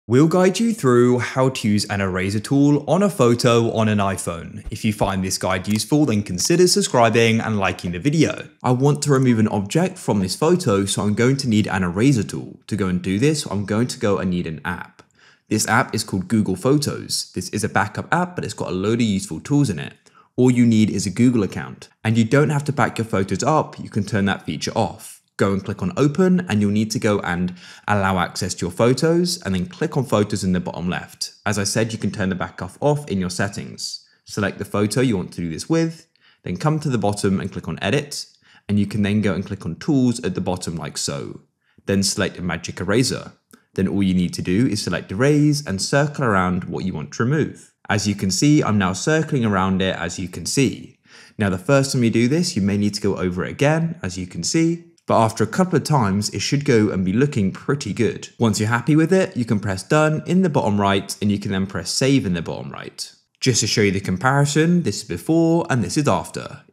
How To Use A Magic Eraser Tool On iPhone - Full Guide
Learn how to use a magic eraser tool on iphone in this video.

GuideRealm